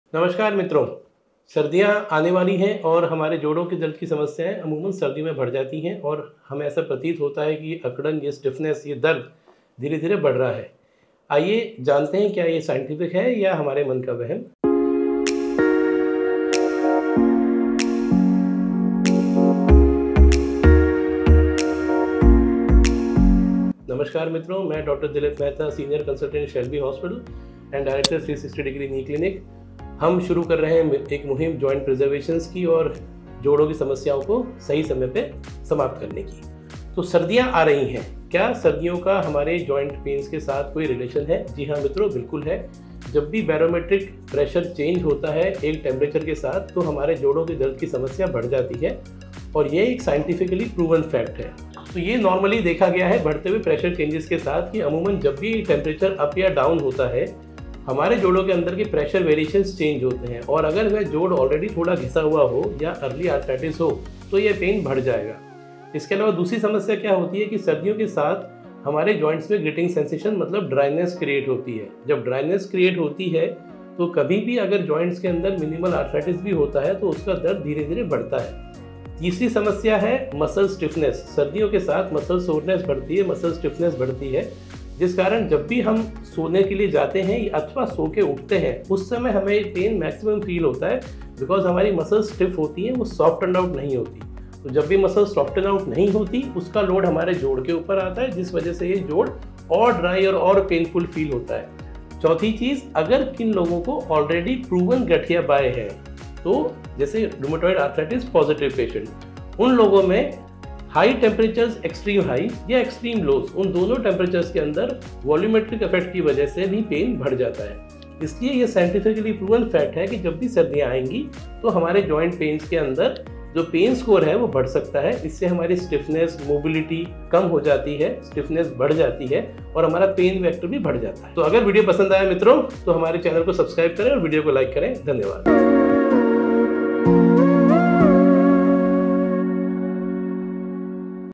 Joint pain in Winter | Dr Dilip Mehta
The leading cause of joint pain in the winter is the chilly, low temperature. Our bodies endure muscle spasms in low temperatures, which can worsen our joints and result in stiffness. Our bodies' pain receptors become more sensitive in the winter, which intensifies our already intense agony.